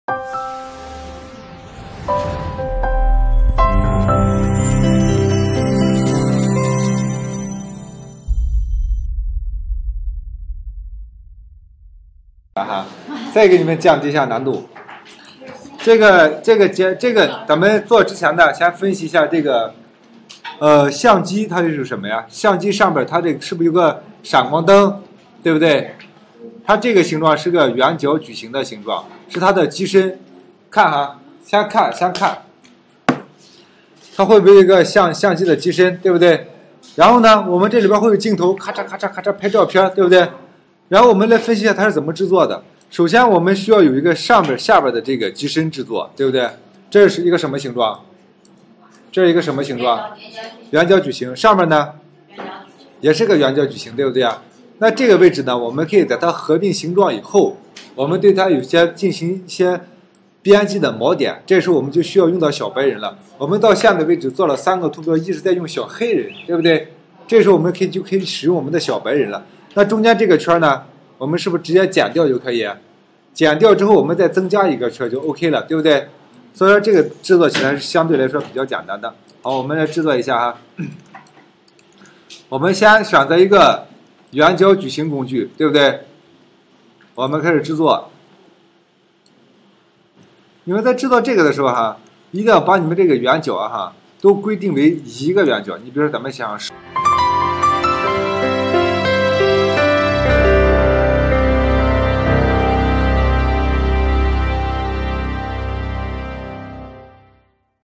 千锋UI教程：46 相机图标制作
【千锋UI】教你玩转图标icon教程（53集）
课程介绍
课程介绍了PS软件的基本使用方法以及剪影图标、轻拟物图标、拟物图标的制作，通过学习可以运用软件制作不同风格的图标，如：剪影图标、扁平图标、拟物图标等。